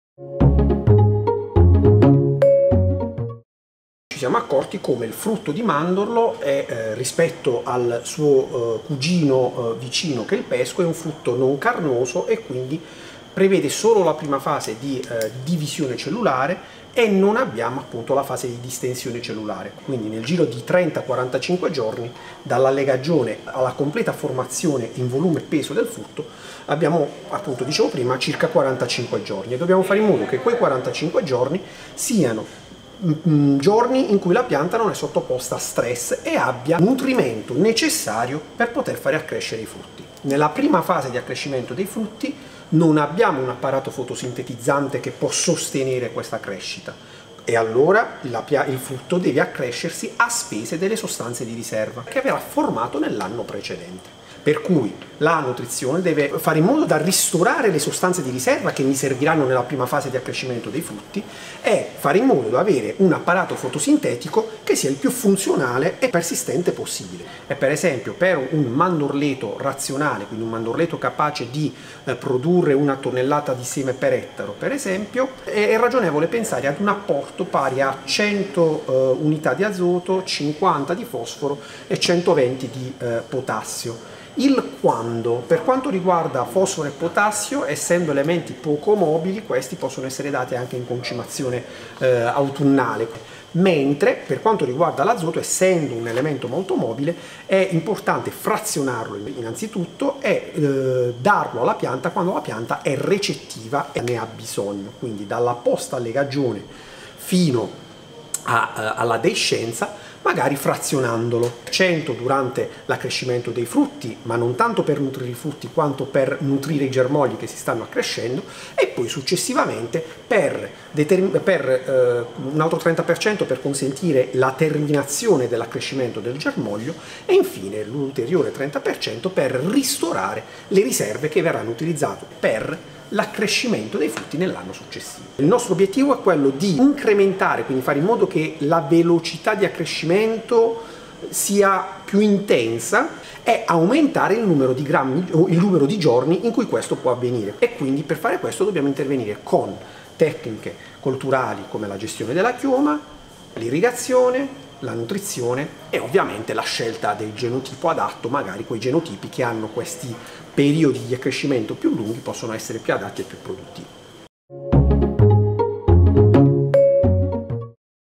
Mandorlo e nutrizione - intervista a Pasquale Losciale
In occasione delle Giornate Tecniche SOI sul mandorlo Fruit Journal ha intervistato il ricercatore Pasquale Losciale del CREA Bari. Lo studioso ha approfondito la questione della nutrizione della frutticola in questione.

Interviste: Teresa Manuzzi
Riprese e montaggio: Michele Lucarelli